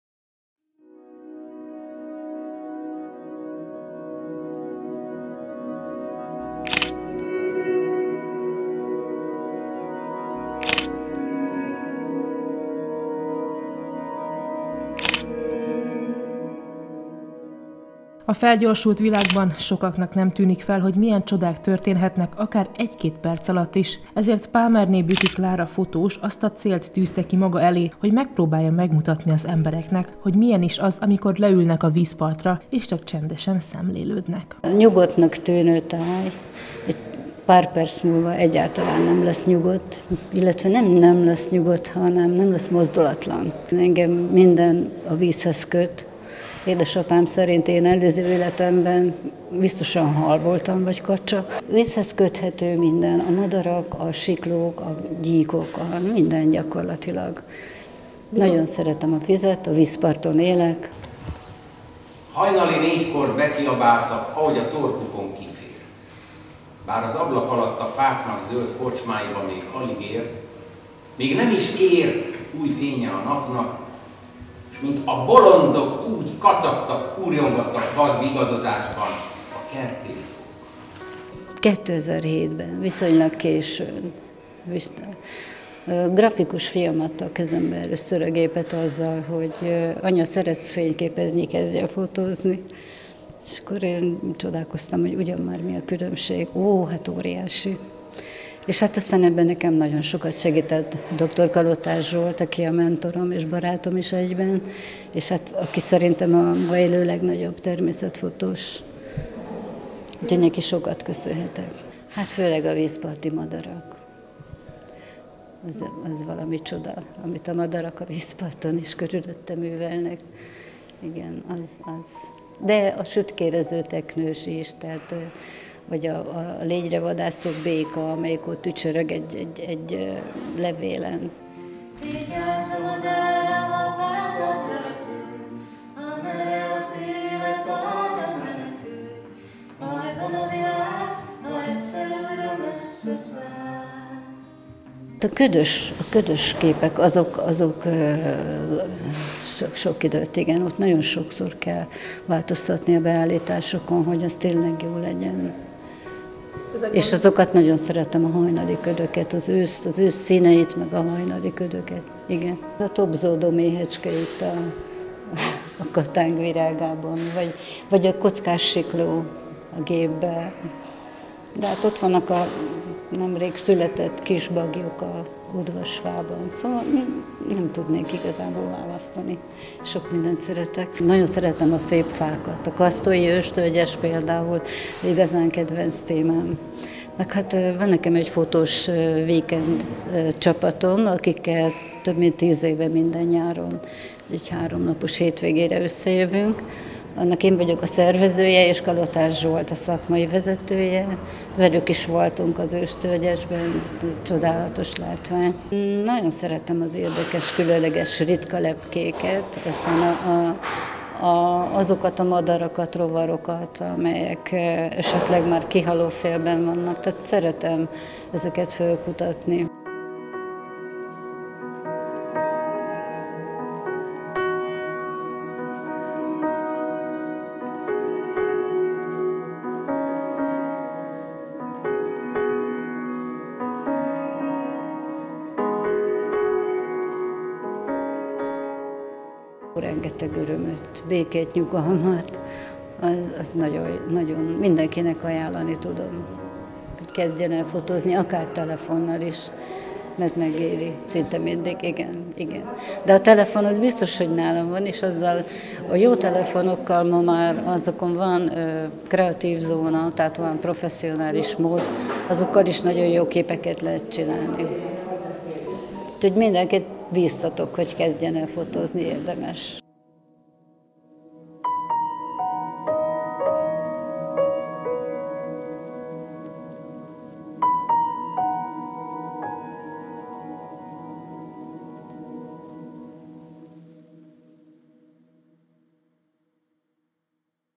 Pámerné Bükky Klára - fotókiállítás, Tolna
Idén Pámerné Bükky Klára "Kis történetek" című fotókiállításával nyitották meg a Bárkafesztivált. A fotós elmondása szerint különösen szoros szálak fűzik a vízparthoz és az ottani élővilághoz.